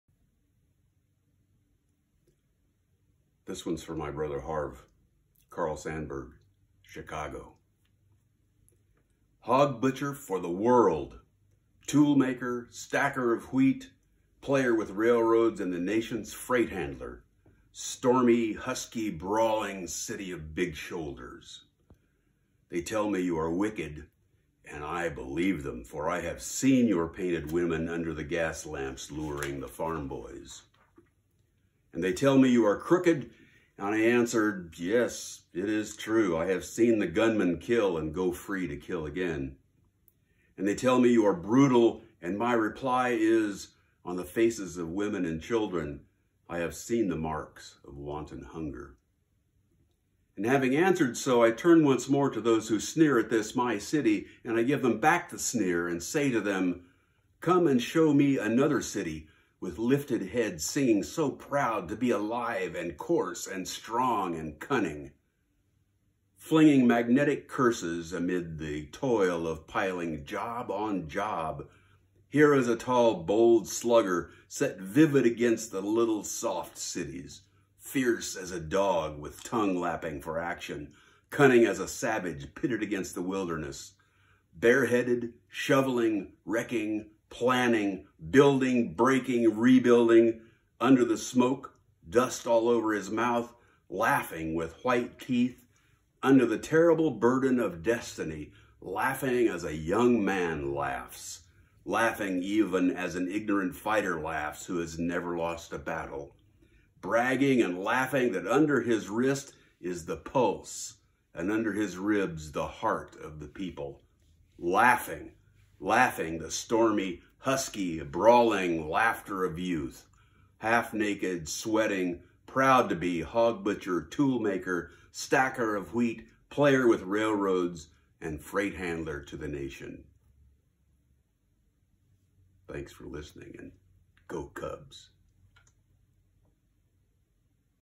Dr Cox Reads Poetry
"Chicago"--Carl Sandburg